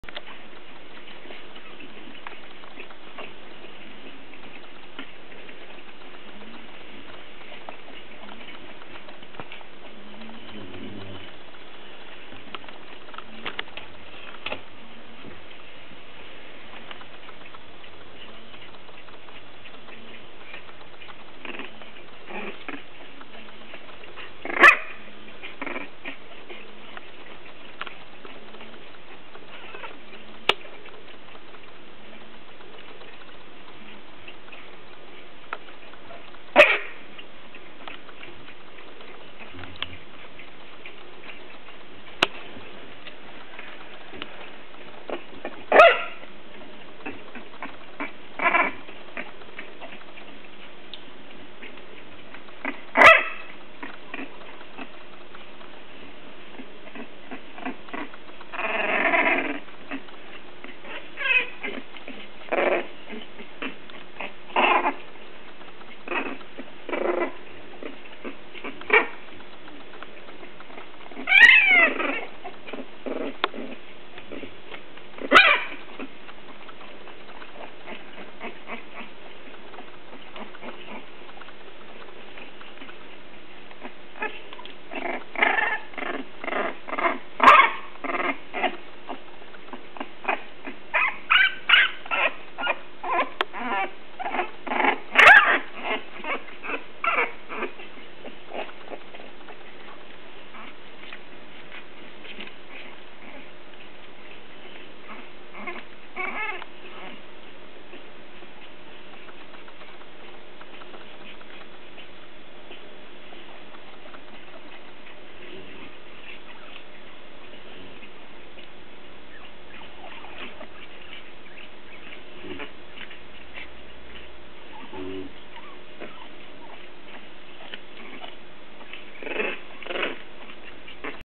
English Bulldog Puppies 3 weeks of Age
English Bulldog puppies playing, litter of 2005 Femme Fatale X Wyecaple Good to Go